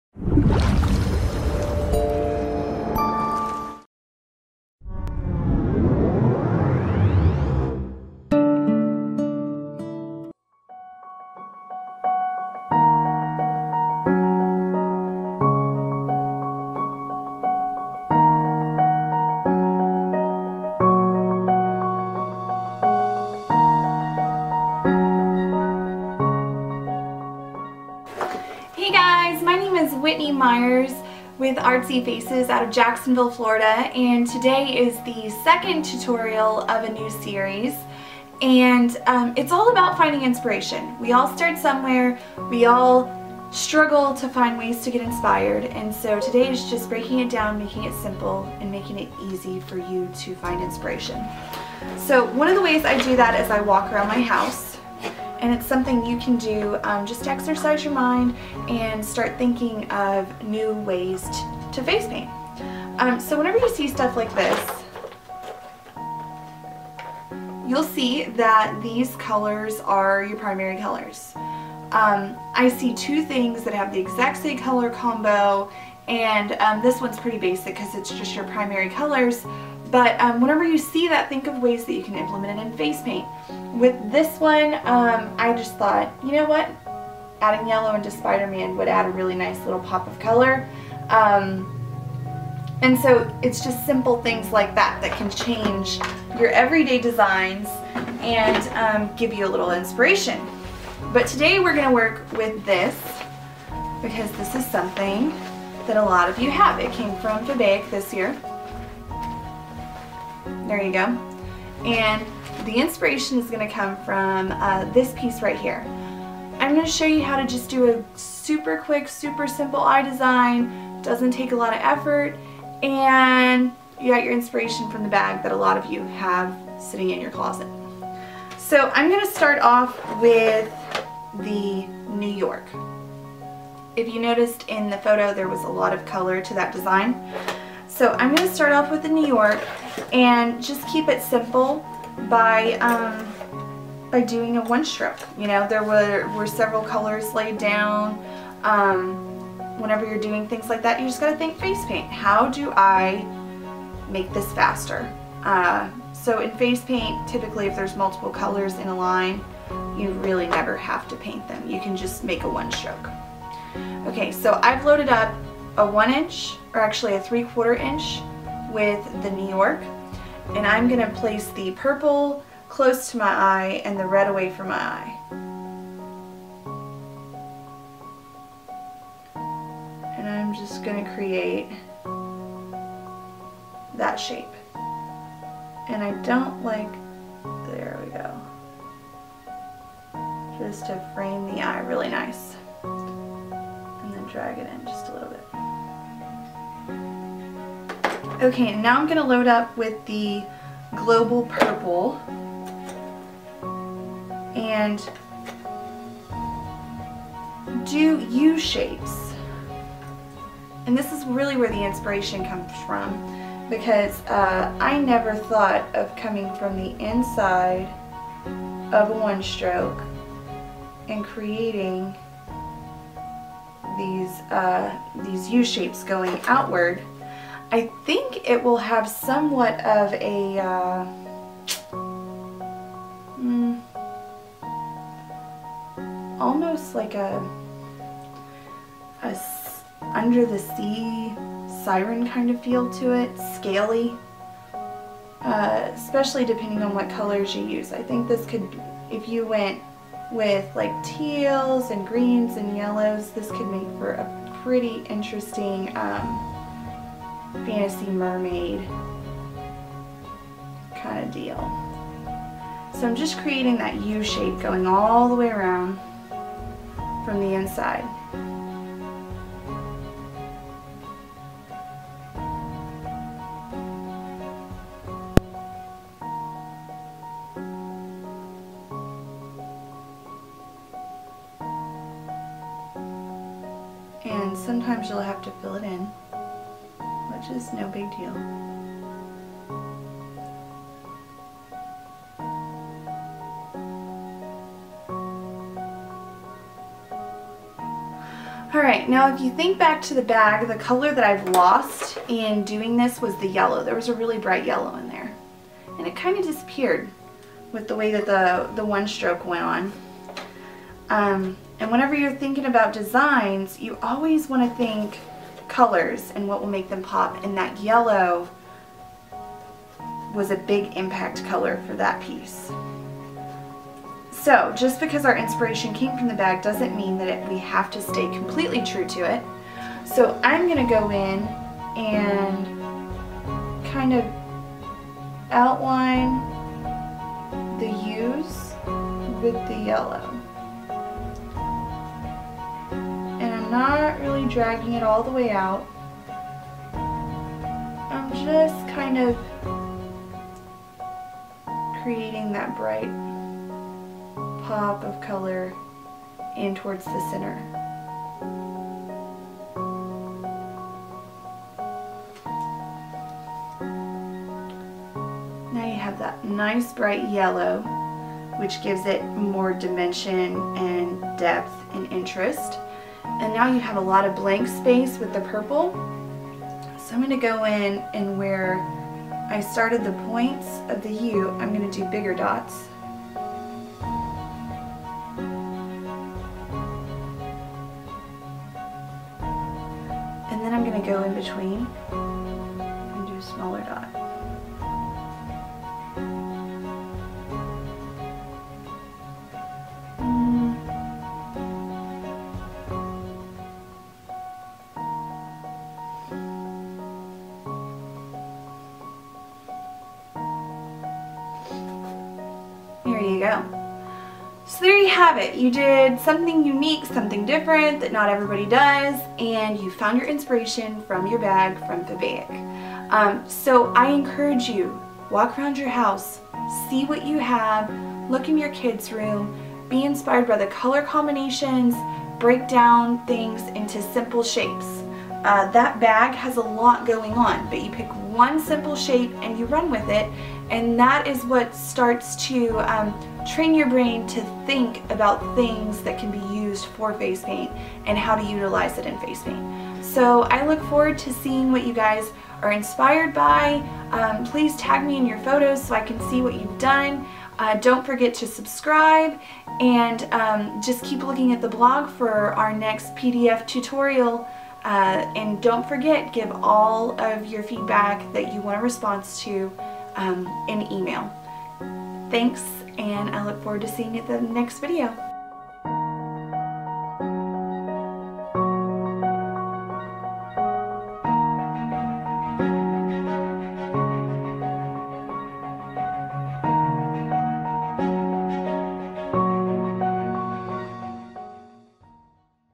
How to face paint tutorial By Whitney Myers
Tutorial by Whitney Myers, Finding Inspiration

Products Used:

Global Colours Fun Stroke New York, Standard Purple, Standard Yellow